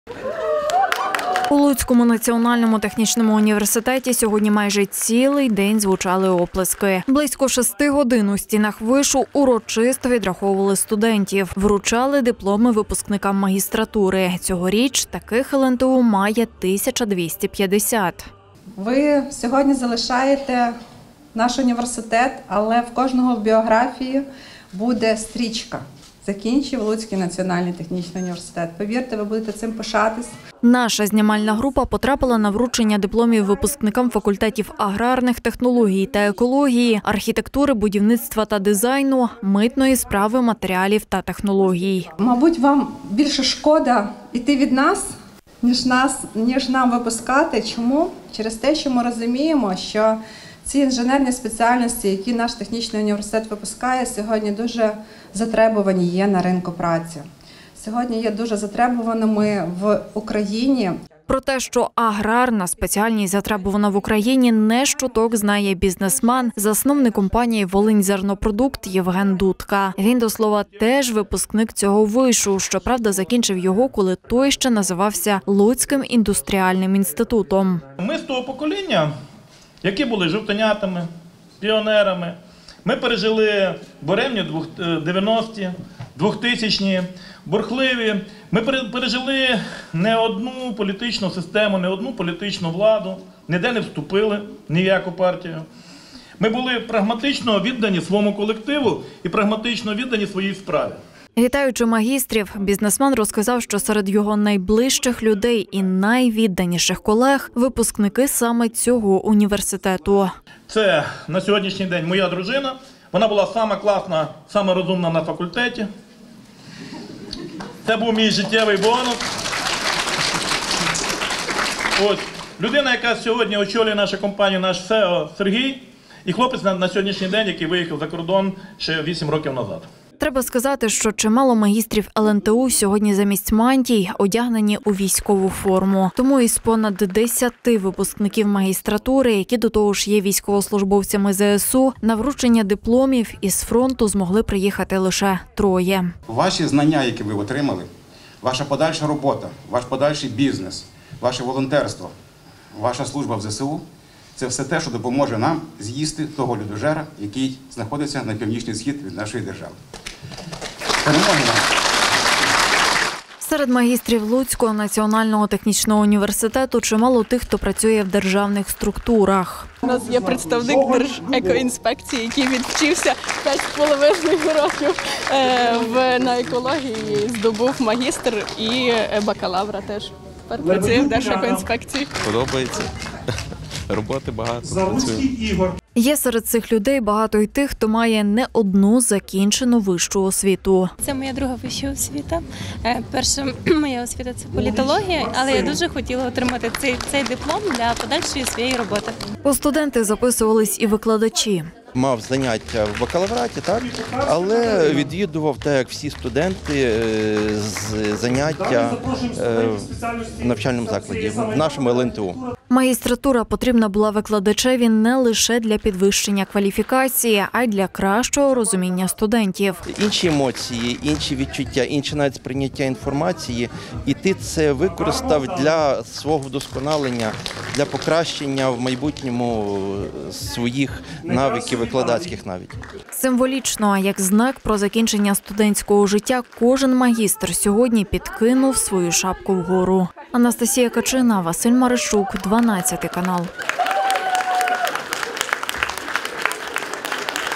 🎓 Мантії, дипломи і оплески. У Луцькому НТУ випустили магістрів
У Луцькому національному технічному університеті сьогодні майже цілий день звучали оплески. Близько шести годин у стінах ВИШу урочисто відраховували студентів – вручали дипломи випускникам магістратури. Цьогоріч таких ЛНТУ має 1 250.